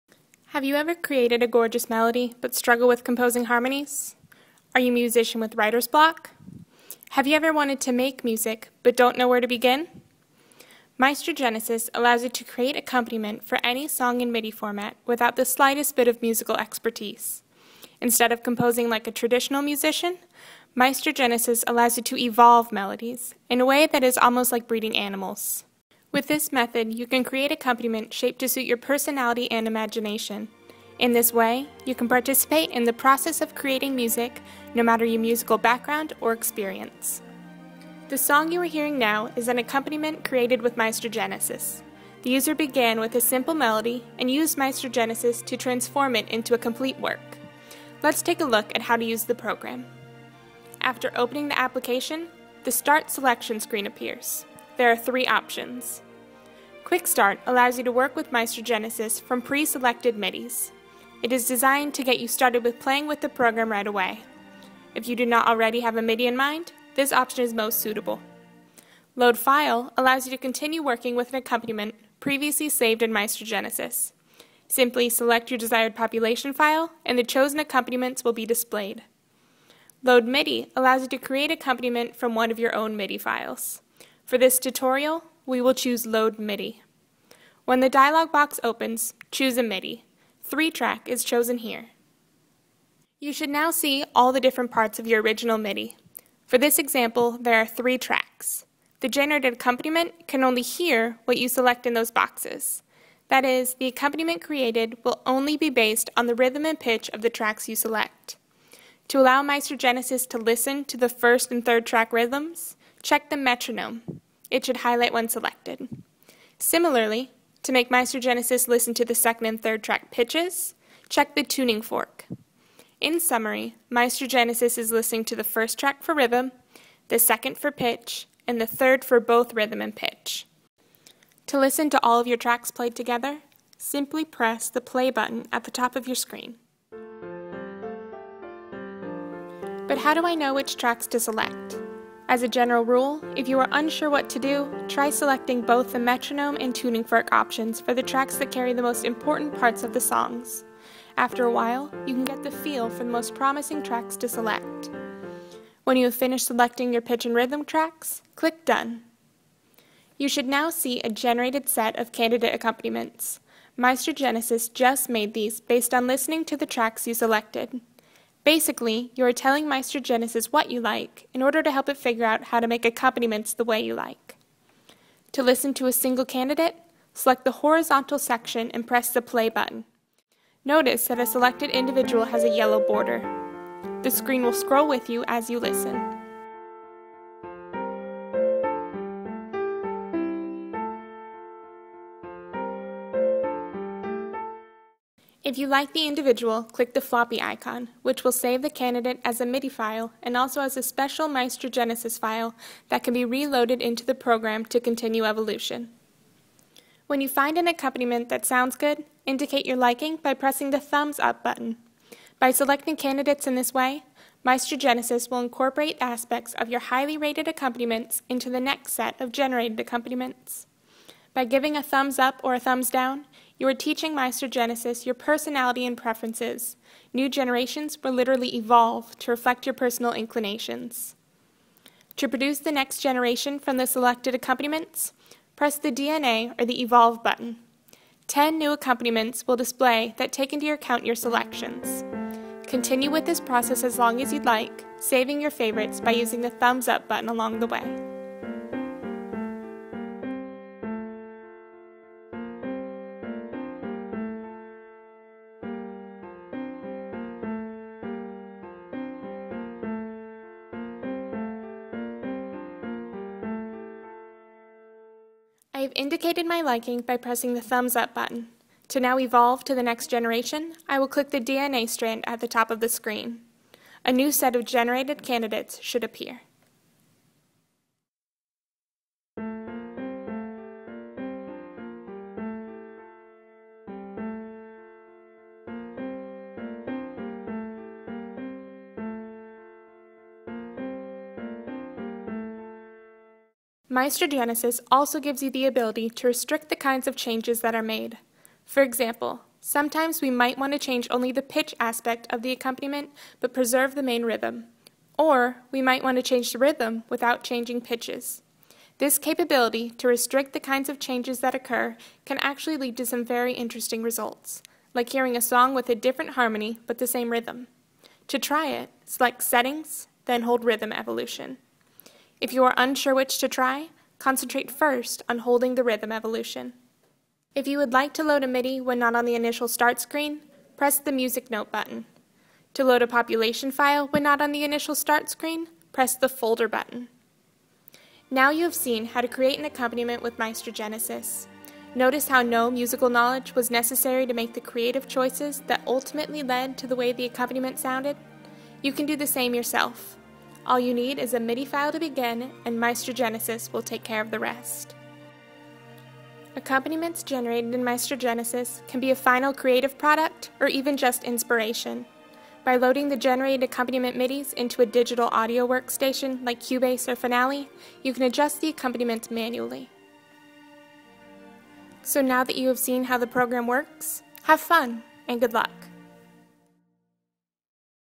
MaestroGenesis Tutorial
allows you to generate accompaniment for existing MIDI compositions through a breeding process similar to animal breeding. The result is that amateur musicians and non-musicians can create accompaniment without any musical expertise.

This tutorial explains the basics of using MaestroGenesis to generate your own unique compositions.